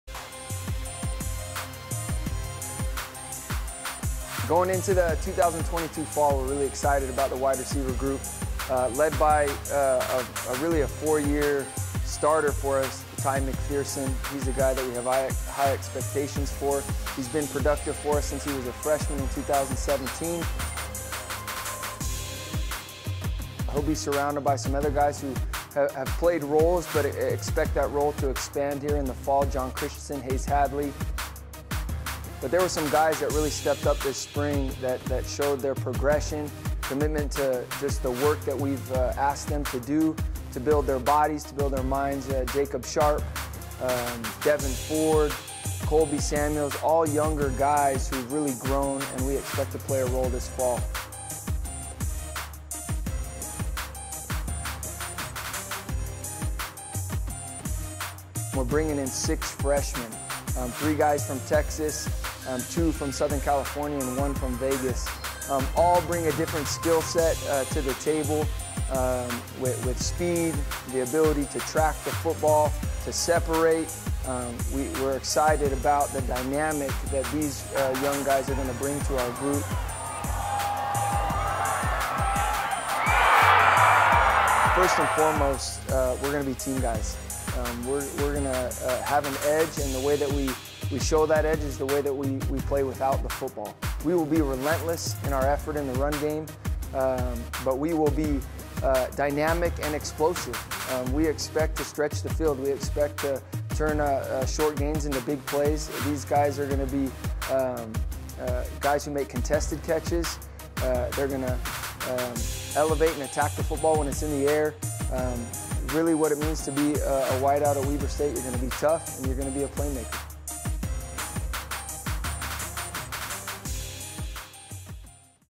2022 Weber State Football position preview - WR
Wildcat assistant coach Skyler Ridley takes a look at the Wildcat wide receivers heading into the 2022 season, led by senior captain Ty MacPherson.